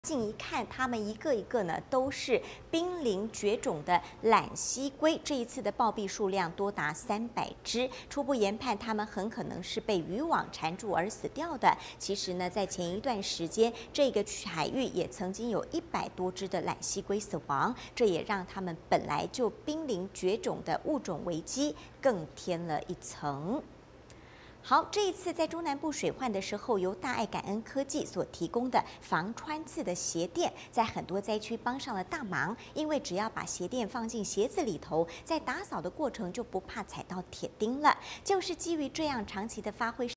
一月兩起！墨300隻瀕危欖蠵龜暴斃
墨西哥南部的海岸，周二漁民發現大量海龜集體暴斃。

海面上出現大量黑色物體，拉近距離一看，全部都是瀕臨絕種的欖蠵龜，數量高達300隻，初步調查是因為被非法漁網纏住而死。欖蠵龜每年六到九月都會到墨西哥太平洋沿岸產卵、繁衍後代，沒想到這次遇上死劫。其實，幾天前，也有1百多隻海龜死亡，但原因還有待調查。